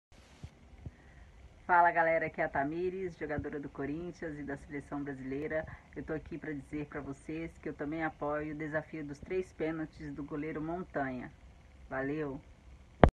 Desafio do Montanha: Tamires (Depoimento)
Tamires Cássia Dias Gomes é uma futebolista profissional brasileira que atua como lateral-esquerda. Ela atua como lateral-esquerda do Corinthians e da Seleção Brasileira de Futebol Feminino. A jogadora foi uma das 23 convocadas para a Copa do Mundo de Futebol Feminino de 2019 pelo técnico Vadão.